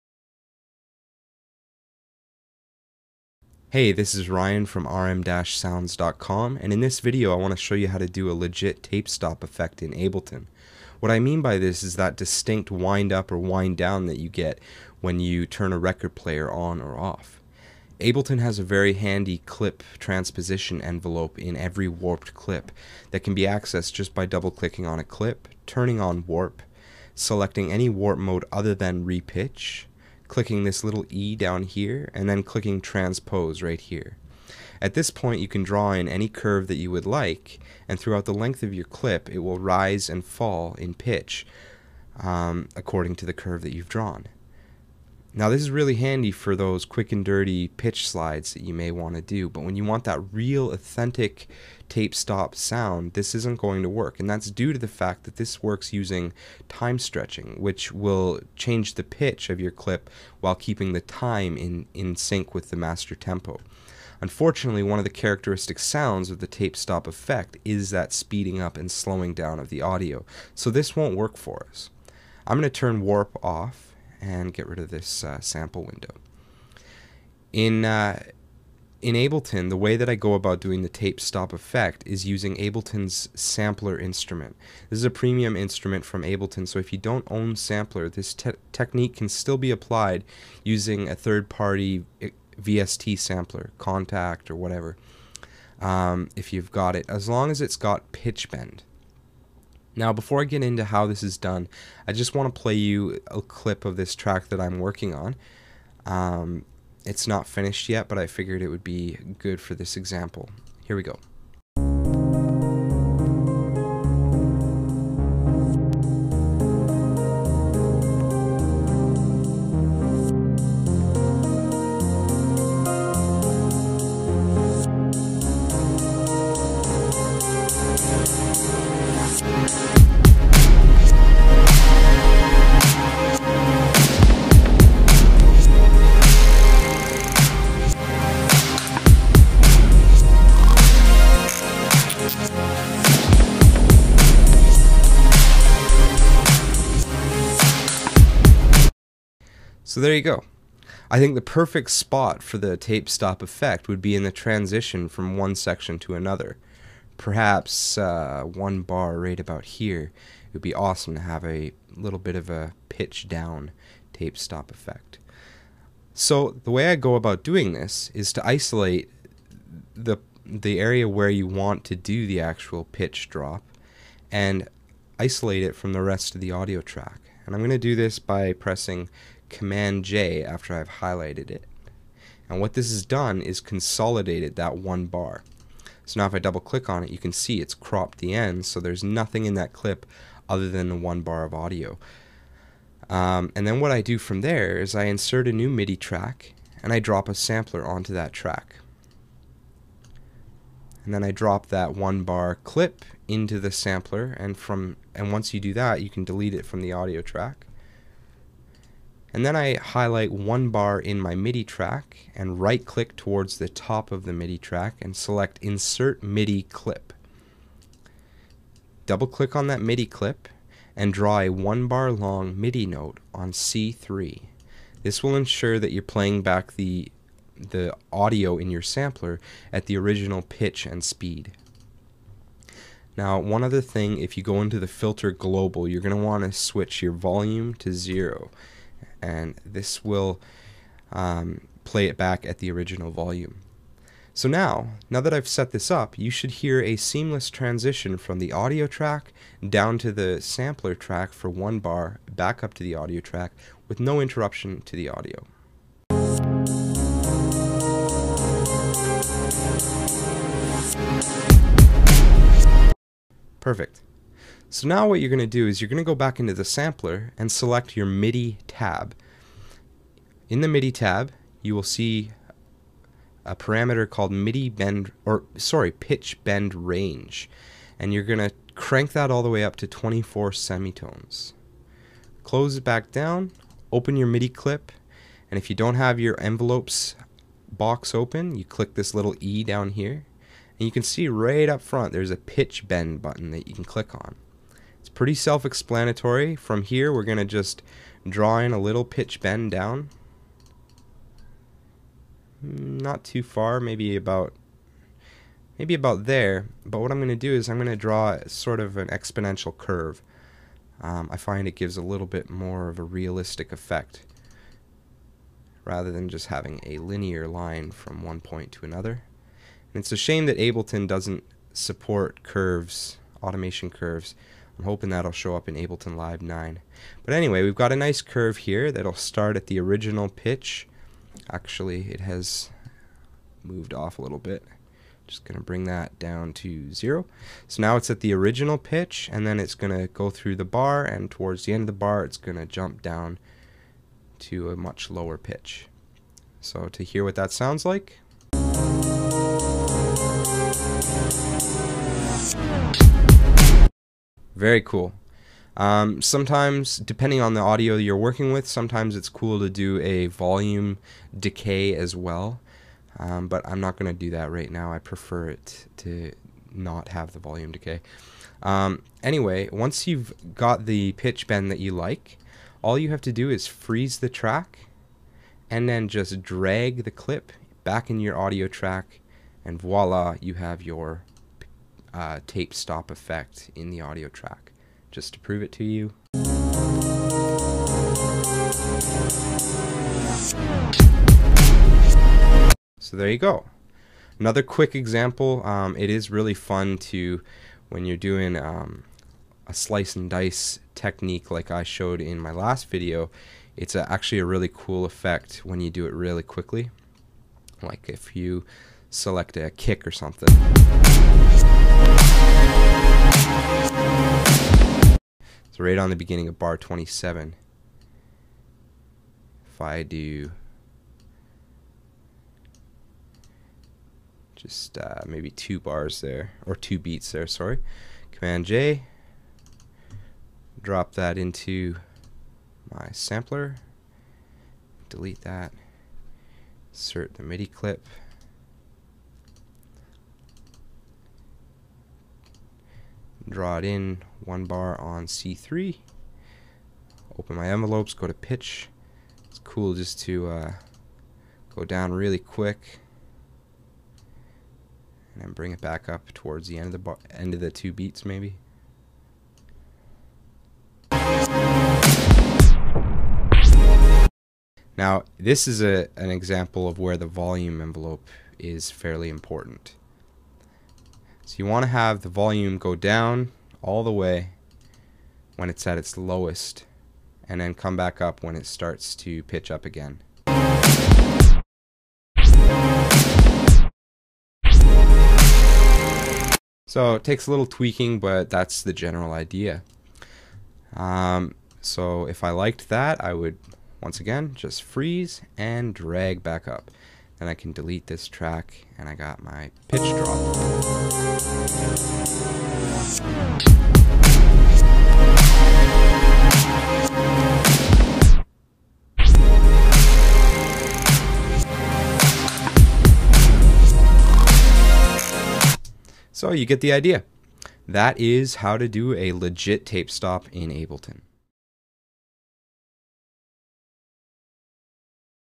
Legit Tape Stop In Ableton
In this video I describe how to achieve a REAL tape stop effect in Ableton Live, without time-stretching! Slow down & pitch down your audio at the same time.